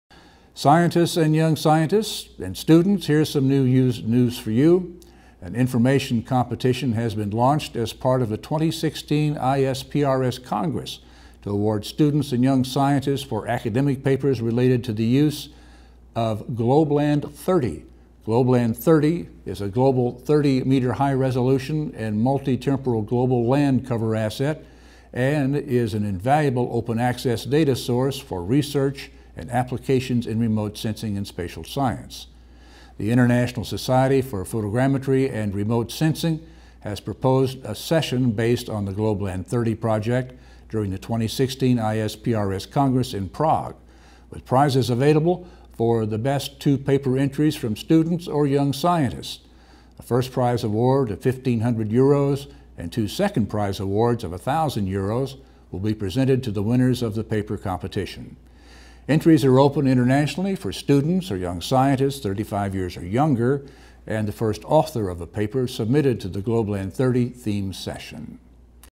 ISPRS competition for land cover studies
Students and young scientists, here’s some news for you. An international competition has been launched as part of the 2016 ISPRS Congress to award students and young scientists for academic papers related to the use of GlobeLand30. GlobleLand30 is a global 30 m high-resolution and multi-temporal global land cover dataset and is an invaluable open-access data source for research and applications in remote sensing and spatial science. The International Society for Photogrammetry and Remote Sensing has proposed a session based on the GlobeLand30 project during the 2016 ISPRS Congress in Prague, with prizes available for the best two paper entries from students or young scientists. A first prize award of 1,500 Euros and two second prize awards of 1,000 Euros will be presented to the winners of the paper competition. Entries are open internationally for students or young scientists (35 years old or younger) and the first author of a paper submitted to the GlobeLand30 Theme Session.